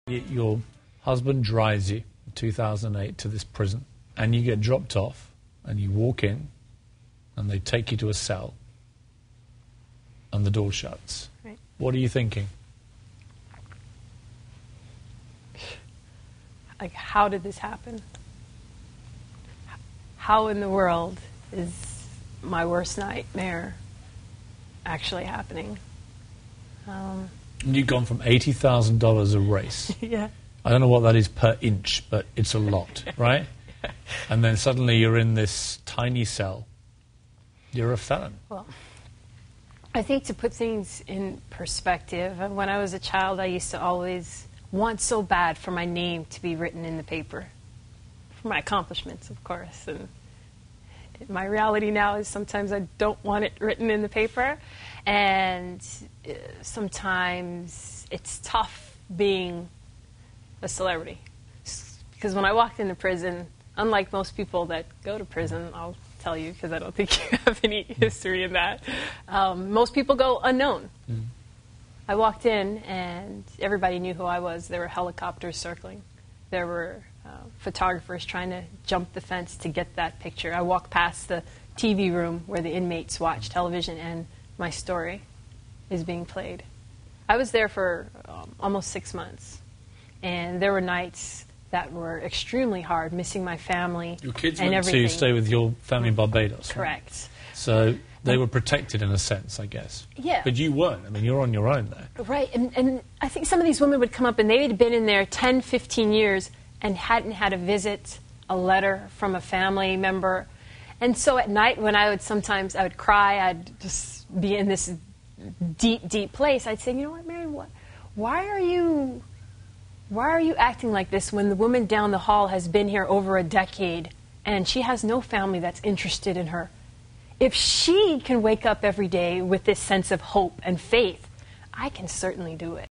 Marion Jones recounts her experience in prison
Marion Jones recounts her experience in prison with CNN's Piers Morgan.